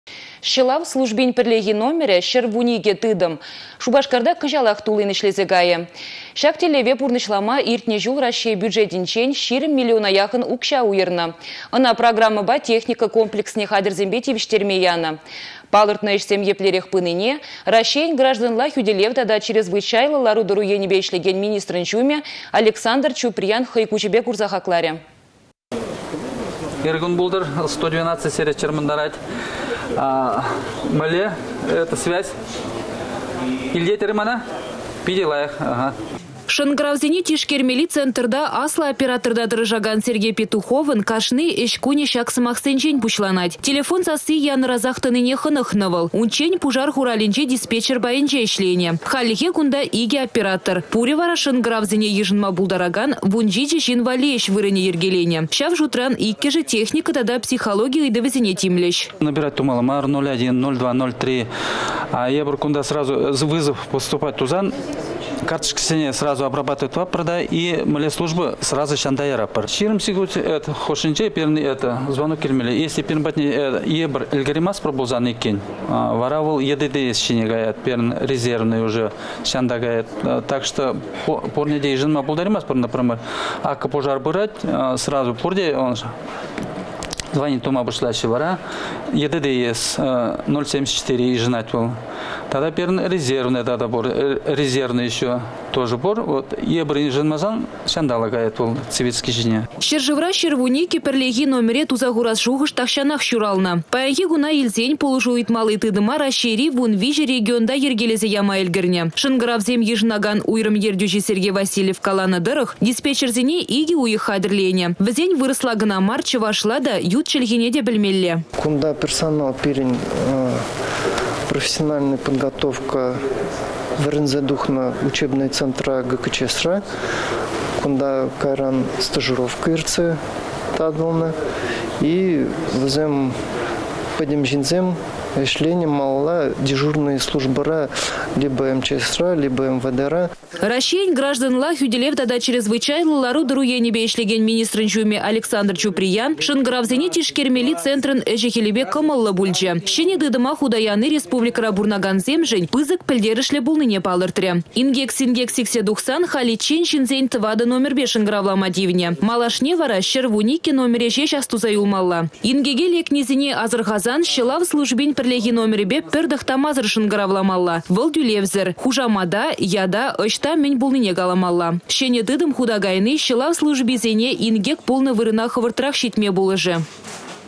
Çăлав службин пĕрлехи номерĕ
Çăлав службин пĕрлехи номерĕ - 112 тытăм Шупашкарта кăçалах туллин ĕçлесе кайĕ. Çак тĕллеве пурнăçлама иртнĕ çул Раççей бюджетĕнчен 20 миллиона яхăн укçа уйăрнă. Ăна программăпа техника комплексне хатĕрсемпе тивĕçтерме янă. Палăртнă ĕçсем еплерех пынине Раççейĕн гражданла хÿтĕлев тата чрезвычайлă лару-тăру енĕпе ĕçлекен министрăн çумĕ Александр Чуприян хăй куçĕпе курса хакларĕ.